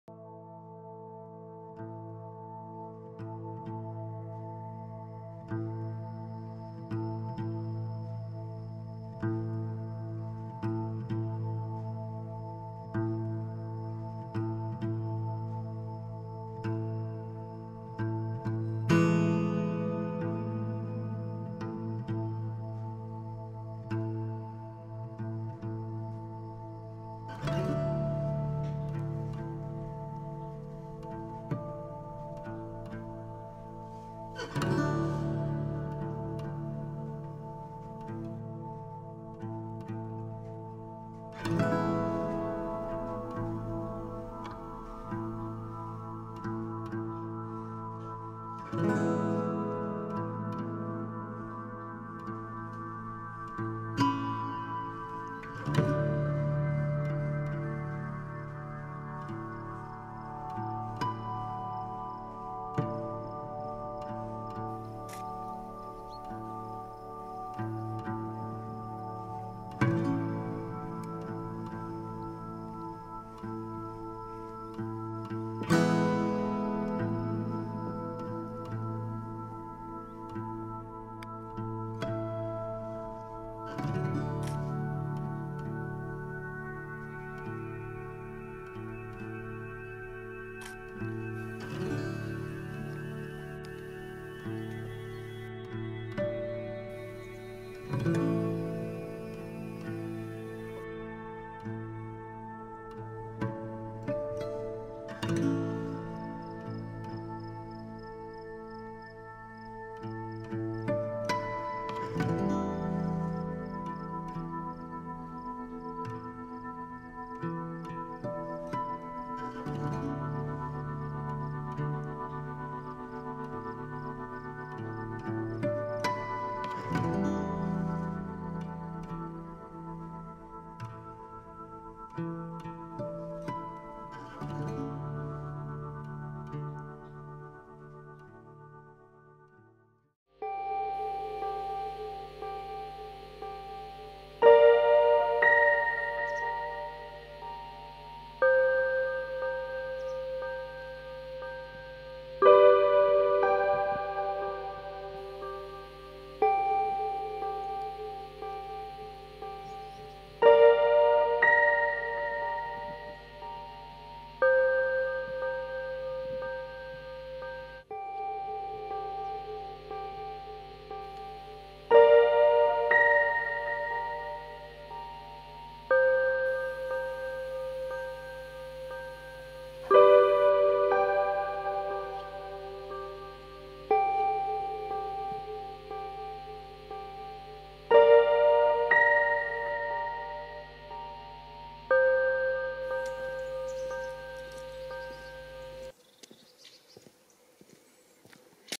Aukštaitijos rūkai // Morning fogs of Aukštaitija
Ankstyvą rytą kelyje su fotografu Algiu Jakštu // Out and about in the early morning with photographer Algis Jakštas.